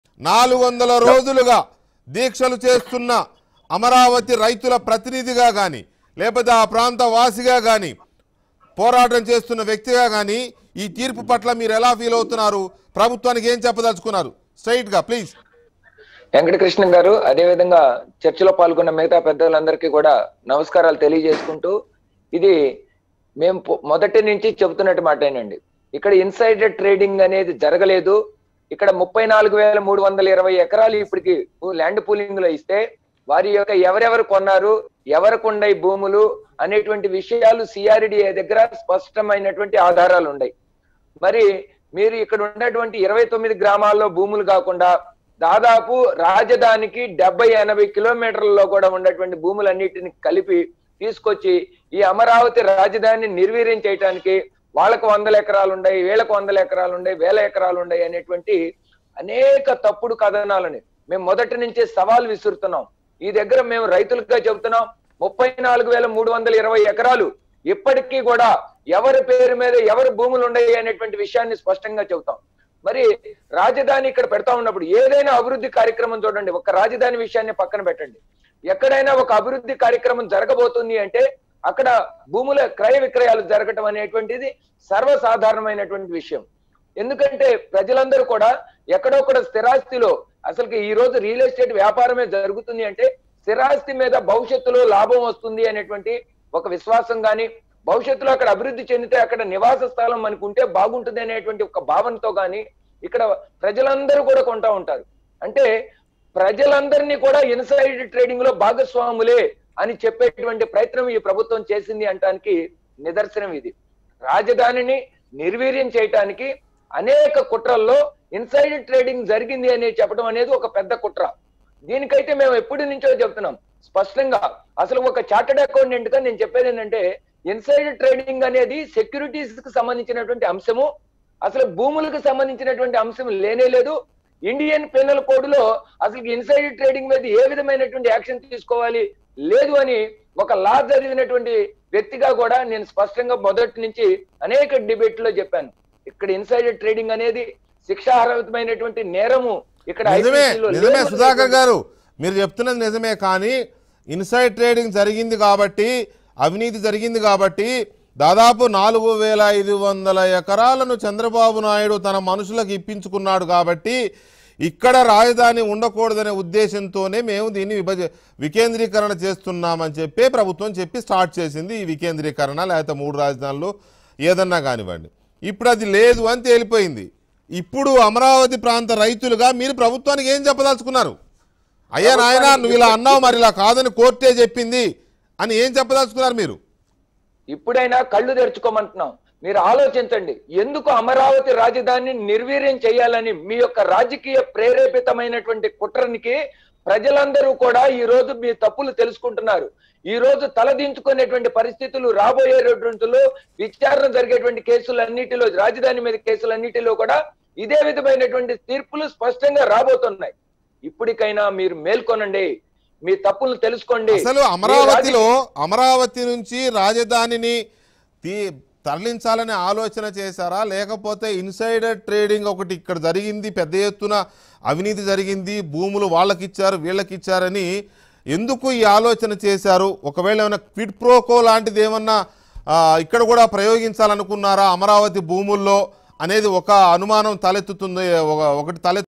Amaravati Farmer Puvvada Sudhakar Reaction | AP High Court on Insider Trading in Amaravati | ABN
AP Capital Issue :  Nara Chandrababu naidu Vs AP CM Jagan
Amaravati Farmers Capital Fight
No Insider Trading In Amaravati: HC
In what can be a setback to the Andhra Pradesh government headed by chief minister Y S Jagan Mohan Reddy, the state high court on Tuesday struck down the criminal cases filed by the state Crime Investigation Department (CID) in connection with the alleged insider trading in land transactions at Amaravati.
The high court gave a judgment to this effect, stating that there was no scope for insider trading in the land transactions.
The CID filed cases against several people closer to Telugu Desam Party stating that they had purchased lands in Amaravati after being privy to the information from the then Chandrababu Naidu government that the area was being developed as the state capital.
The state government argued that the leakage of information to a select few people about the capital city amounted to insider trading. However, one of the buyers of the land Kilaru Rajesh moved the high court challenging the CID cases.
He alleged that there were no complaints from anybody who had sold their lands.
“How can the government file the cases without any complaint from the sellers? It is nothing but witch-hunting by the Jagan government,” advocate Posani Venkateshwarlu, who argued on behalf of the petitioner said.
The high court, which heard arguments from both sides that sections under the Indian Penal Code were not applicable to insider trading. It struck down the cases filed by the CID.
It remains to be seen whether the Jagan government will move the Supreme Court or withdraw the cases.
If the case is struck down even in the Supreme Court, then even former advocate general Dammalapati Srinivas and daughter of the present Supreme Court judge would also get big relief.